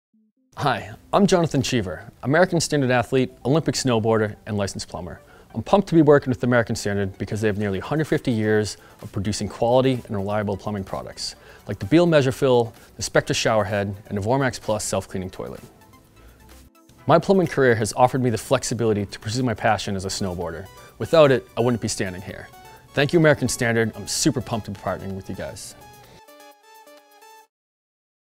Introducing American Standard Athlete and Licensed Plumber Jonathan Cheever
Jonathan Cheever, Olympic Snowboarder, and Licensed Plumber partners with American Standard, creator of truly innovative products like the ActiClean self-cleaning toilet, Spectra shower head, and more. His career in plumbing allows him the flexibility to pursue his passion for snowboarding.